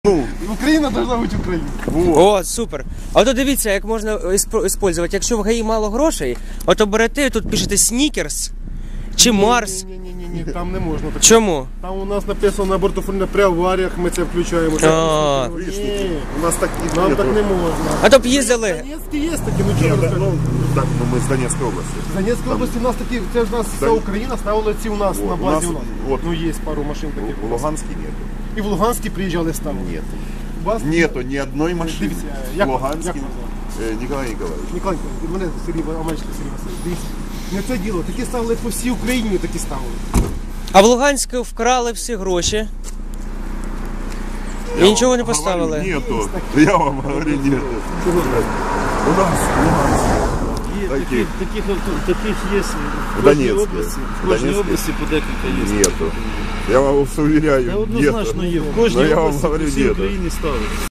ГАИ Киев о digital display на патрульных авто ДПС Луганск
Киевское ГАИ уверяет, что луганские машины ДПС должны быть оснащены цифровыми дисплеями